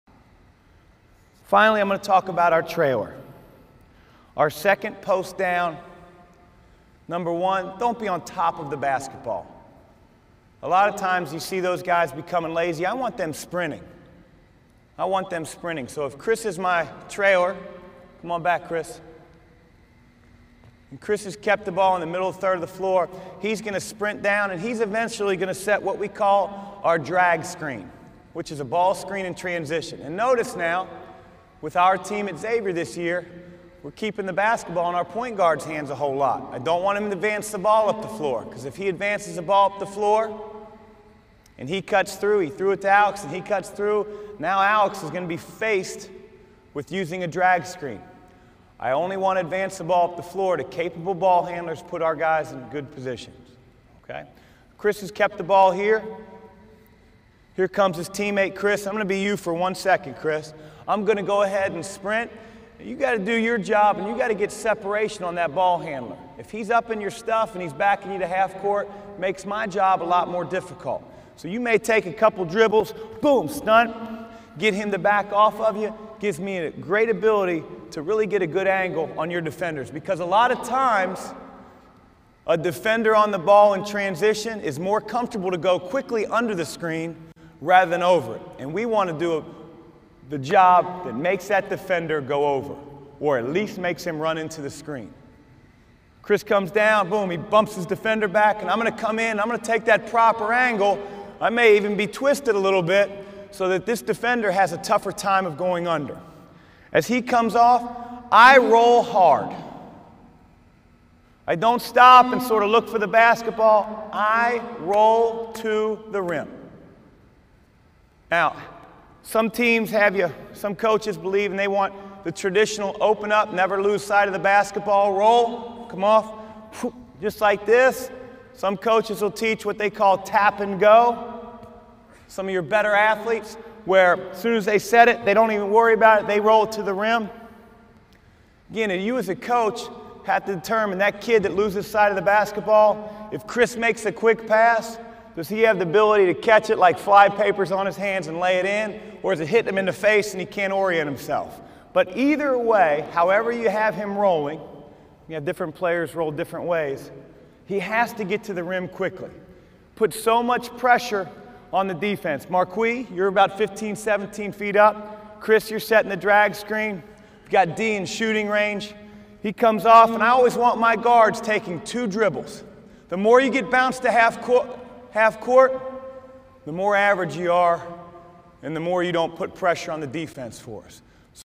Chris Mack: Winning with a Structured Fast Break
2009-10 Basketball Times Rookie Coach of the Year, Chris Mack, reveals some of the principles and rules of this structured fast break. Coach Mack wants his players to have a structure in getting these quick baskets to make it easier for them. This transition offense, when ran by the Suns, is done on both missed and made shots.